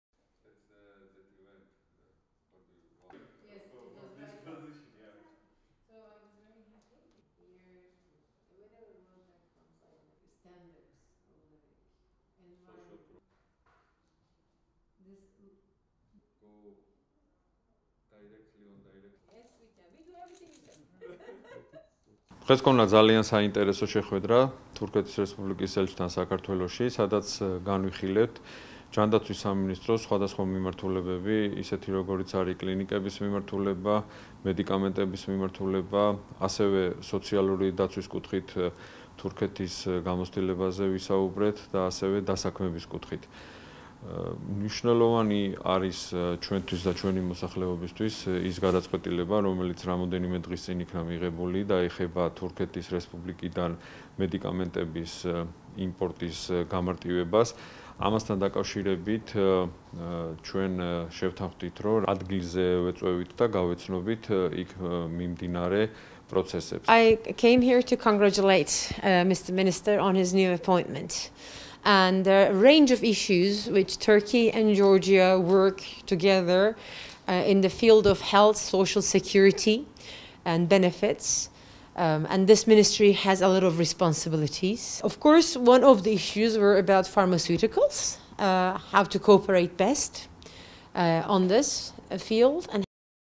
ზურაბ აზარაშვილი ფატმა ჯერენ იაზგანმას შეხვდა
საქართველოს ოკუპირებული ტერიტორიებიდან დევნილთა, შრომის, ჯანმრთელობისა და სოციალური დაცვის მინისტრი  ზურაბ აზარაშვილი  თურქეთის რესპუბლიკის ელჩს საქართველოში ფატმა ჯერენ იაზგანმას შეხვდა.
მხარეებმა მიმდინარე თანამშრომლობის საკითხები და სამომავლო გეგმები განიხილეს. საუბარი შეეხო თურქეთიდან საქართველოში  მაღალი ხარისხის GMP სტანდარტების მედიკამენტების შემოტანას, რაც წამლის ღირებულებას მნიშვნელოვნად შეამცირებს.  შეხვედრაზე მხარეებმა covid 19- ის მართვის მექანიზმებზე იმსჯელეს.
ზურაბ აზარაშვილმა თურქეთის ელჩს საქართველოში, ჯანდაცვის სფეროში მიმდინარე და განხორციელებული პროექტების აქტიური მხარდაჭერისთვის მადლობა გადაუხადა. თავის მხრივ, ფატმა ჯერენ იაზგანმა სამომავლო თანამშრომლობისთვის მზადყოფნა გამოხატა.